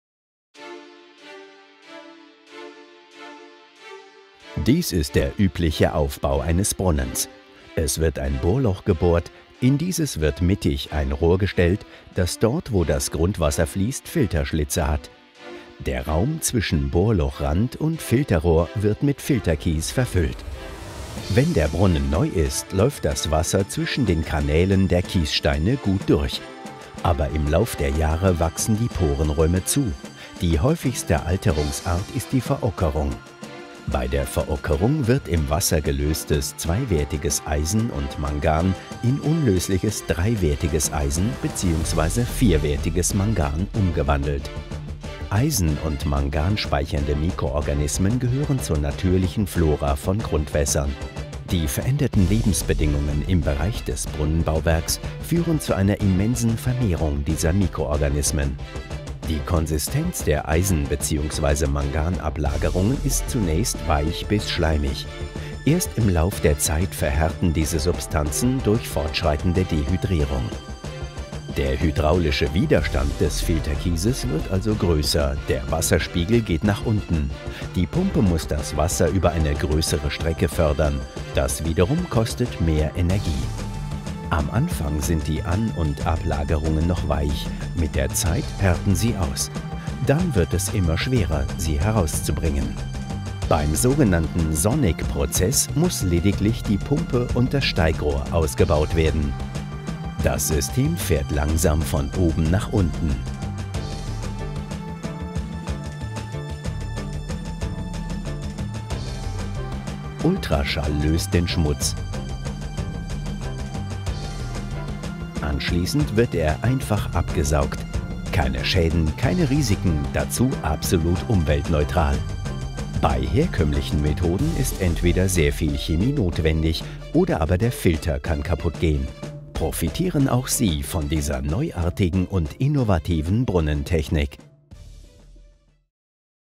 Animationsfilm Sonic Umwelttechnik - by Fränkische Nachrichten
Dieser Animations-Film zeigt die innovative Brunnentechnik der Firma Sonic Umwelttechnik.